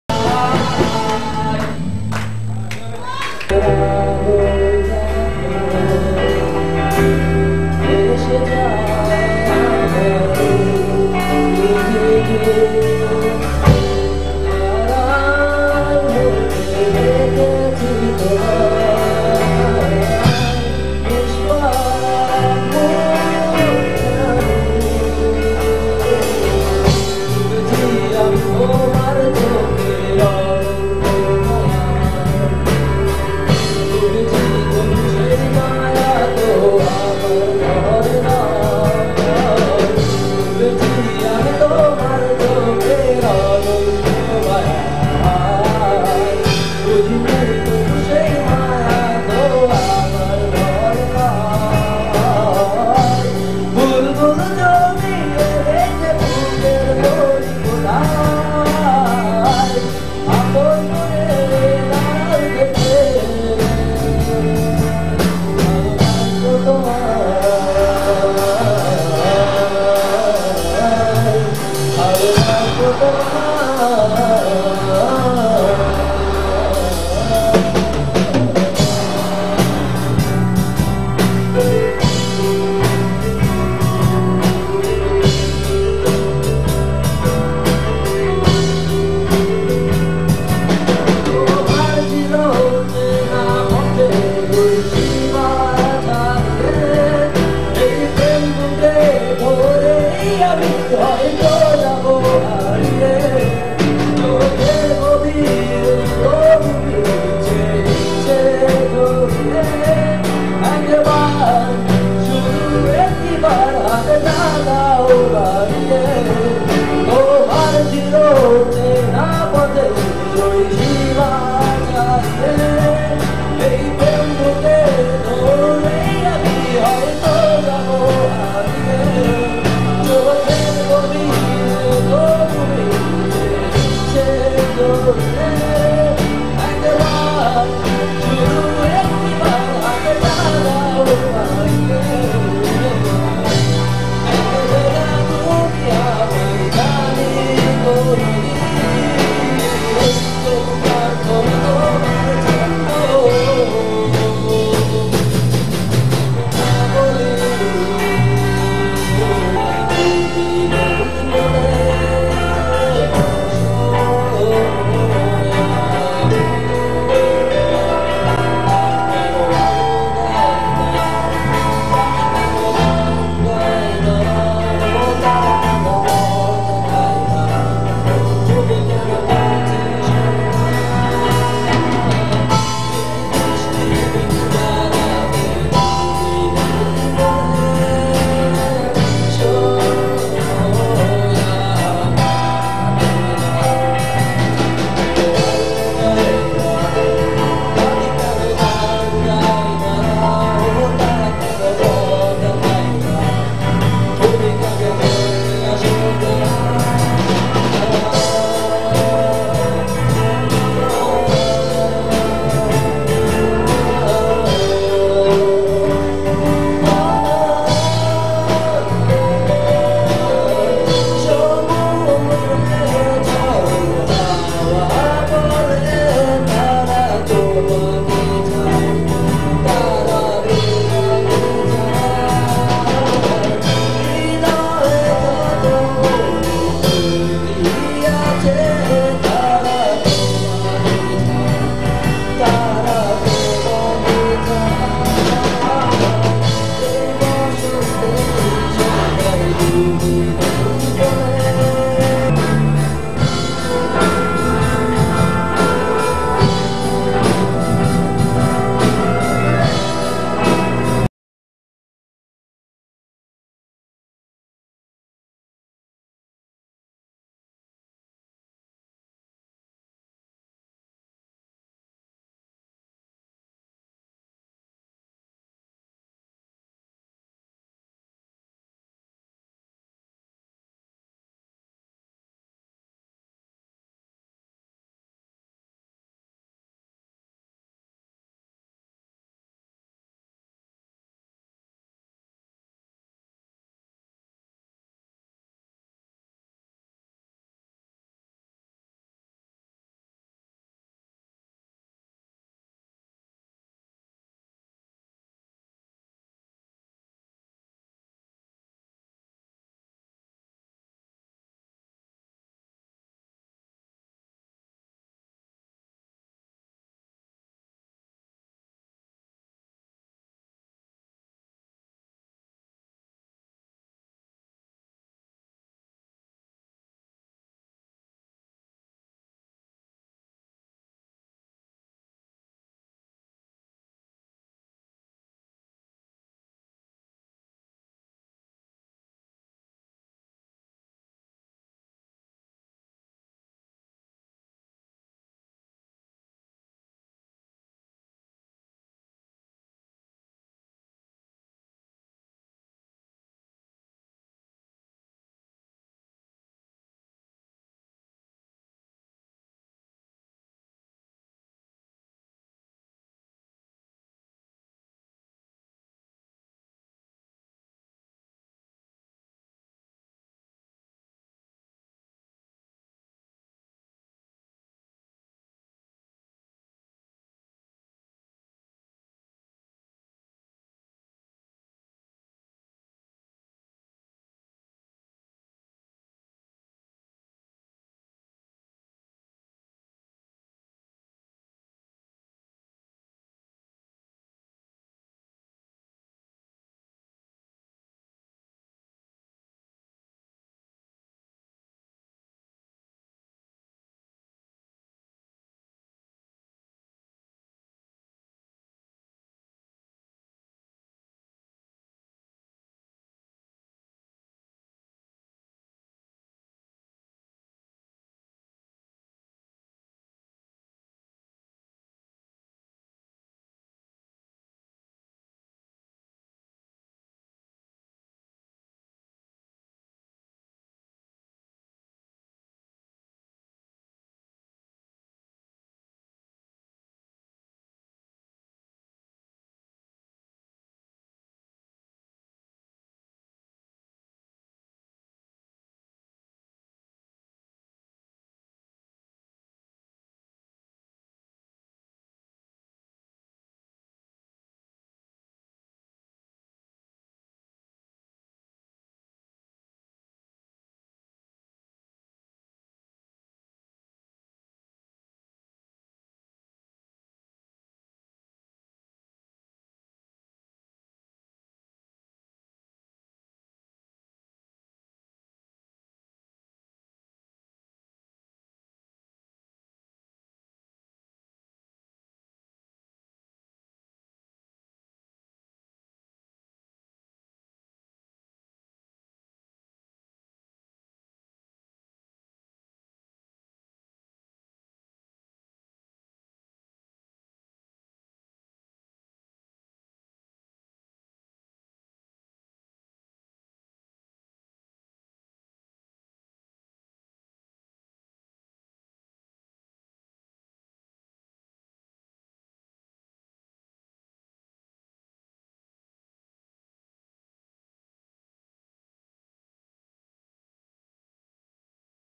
RAG DAY SONG
A Song sung by me Instantly for my friends in my Rag Day, December 2013